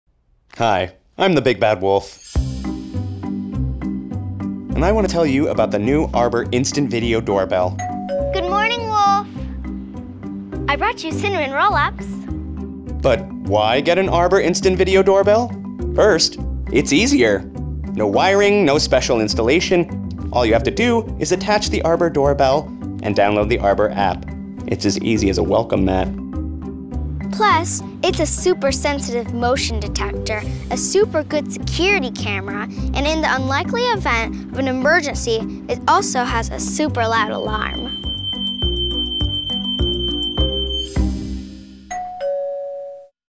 Little Red Riding Hood shows off the Arbor Video Doorbell
Features include an “instant” picture, easy installation, and the most reliable connectivity on the market. Best of all, it works straight out of the box. All you need is a door. Sign up for early access.